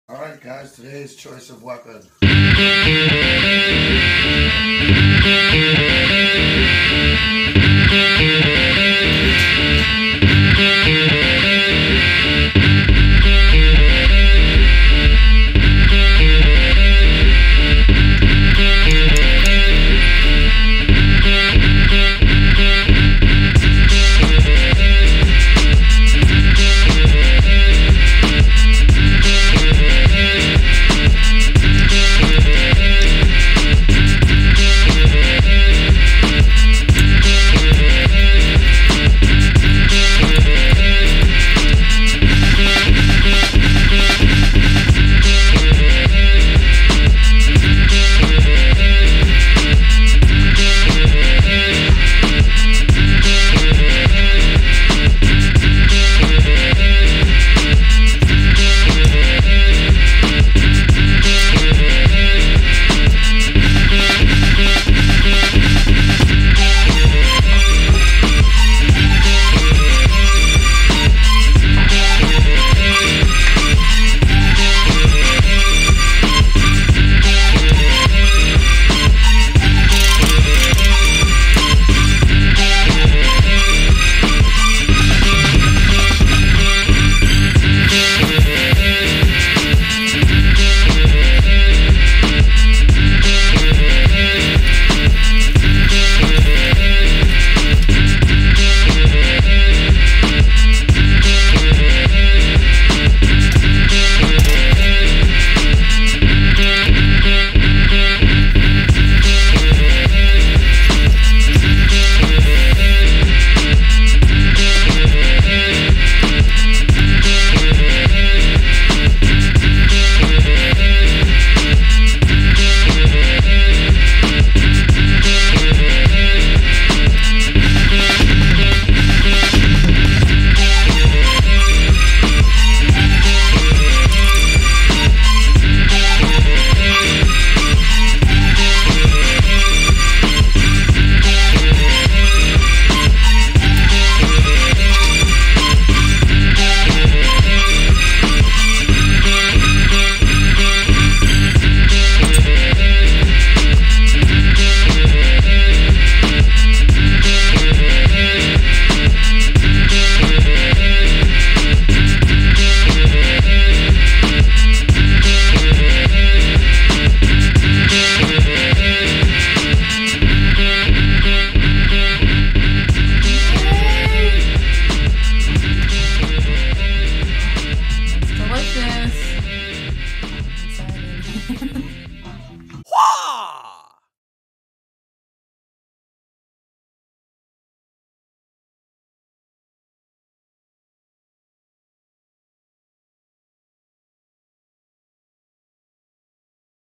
Kung Fu Chef 1 Pulled Chicken Mole Quesadillas
This is the first of a soon to come cooking series by the Kung Fu Chef. LOL. I was never into cooking until I found out a secret on how you can learn to cook easily and immediately. and do it like a 5 star chef.

Each meal feeds 2 people (so you can get your groove on). Hope to hear from you.

Respectfully,

Want to learn Self Defense - YouTube.com/JingShenKuoshu